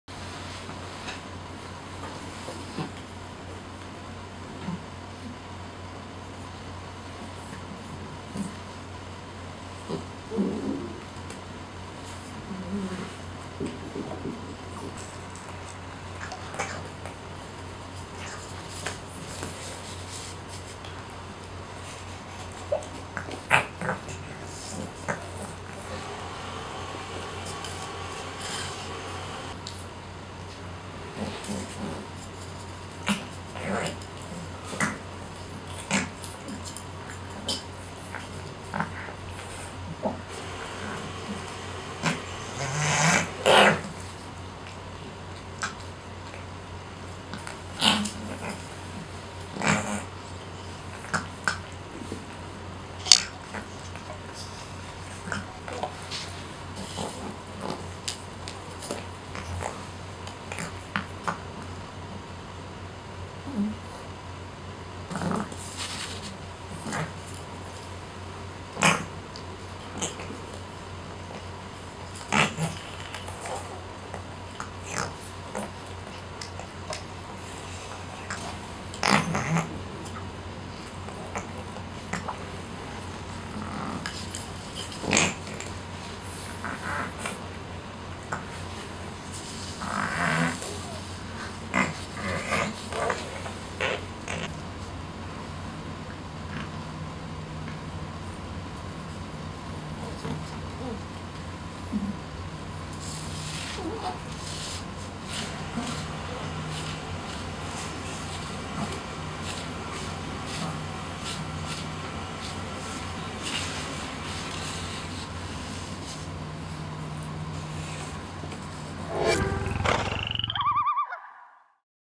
Going "Fishing" for Fluffy Pudge the Cat
Pudge plays with her favorite toy, an octopus on a stick... A game I like to call, "fishing for Pudge".

Proceeds from every sale are donated to local animal charities and rescue organizations.